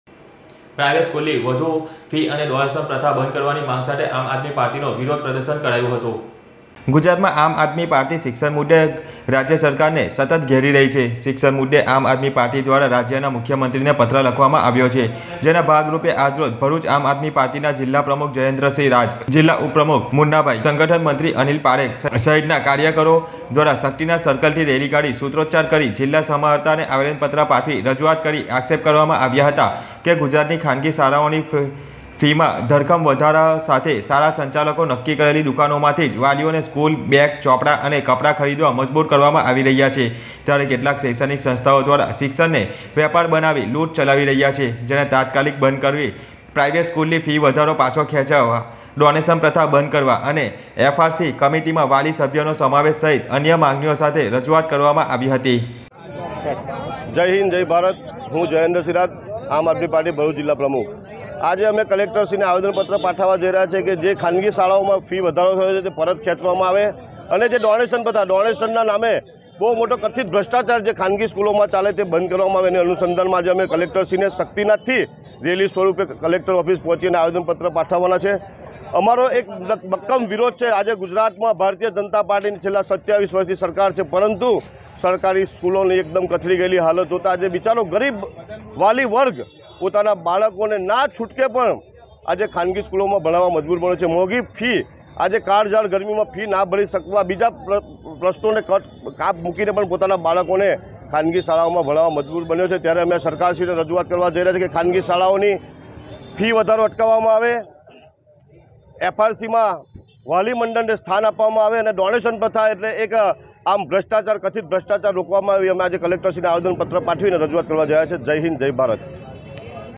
માઁ ટીવી 20.04.2022 પ્રાઇવેટ સ્કૂલ ની વધુ ફી પ્રથા બંધ કરવાની માંગ સાથે આમ આદમી પાર્ટી નો વિરોધ
આપ નો વિરોધ : પ્રાઇવેટ સ્કૂલ ની વધુ ફી અને ડોનેશન પ્રથા બંધ કરવાની માંગ સાથે આમ આદમી પાર્ટી નો વિરોધ પ્રદર્શન.
શક્તિનાથ સર્કલથી રેલી કાઢી જિલ્લા સમાહર્તા ને રજૂઆત કરી : ખાનગી શાળા દ્વારા ફ્રી વધારા પાછો ખેંચવા સહિતની માંગણી સાથે રજૂઆત